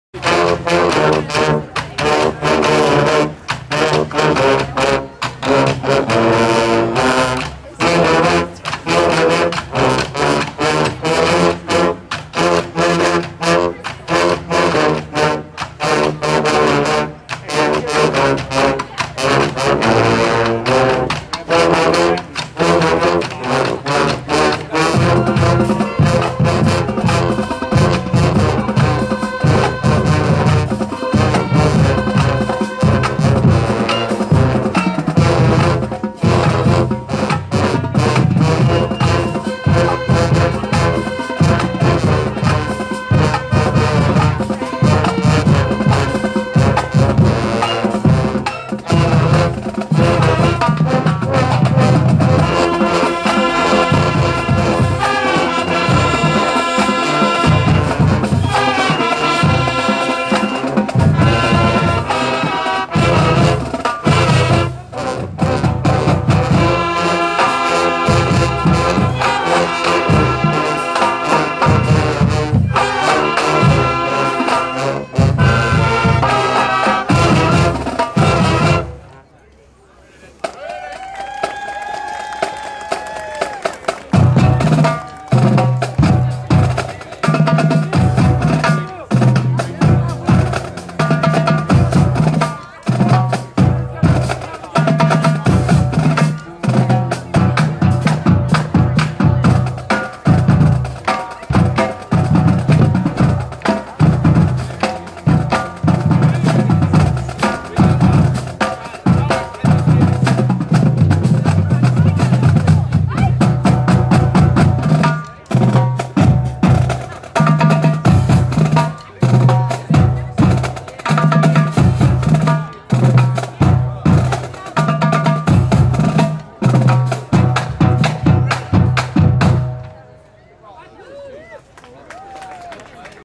Dunwoody HS Band Light Up 3
Dunwoody High School Band at Dunwoody Light Up Festival